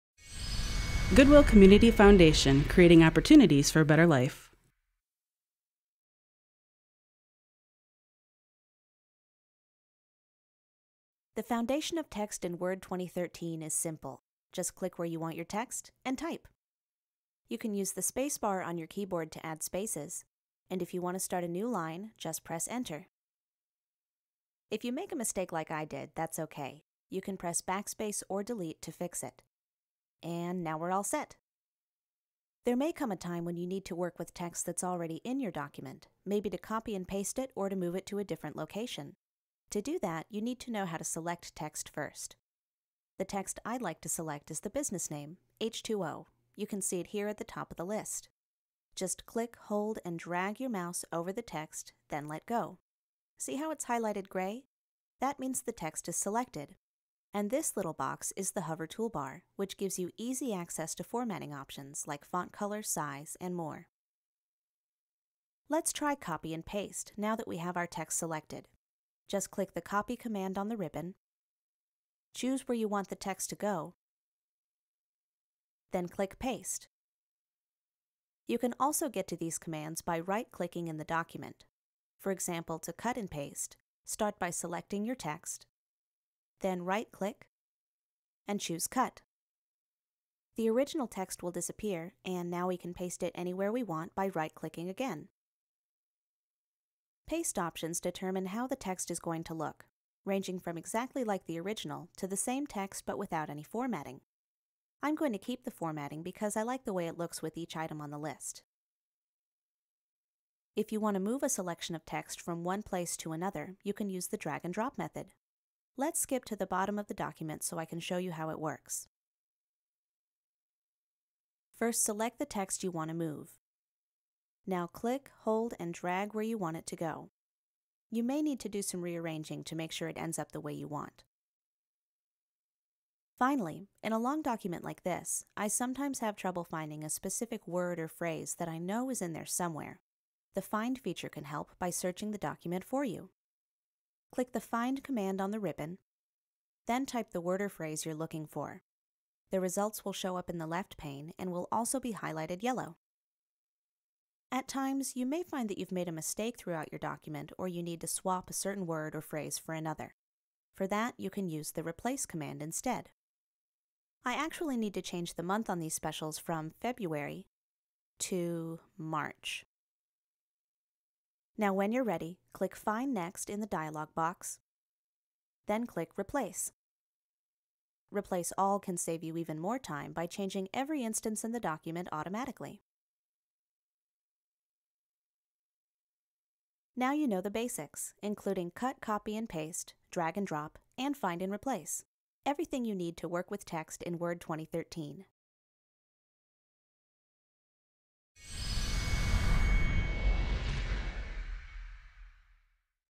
Word 2013: Text Basics
In this video, you’ll learn the text basics of Word 2013. for our text-based lesson.

This video includes information on:
• Selecting and deleting text
• Copying and moving text
• Using Find and Replace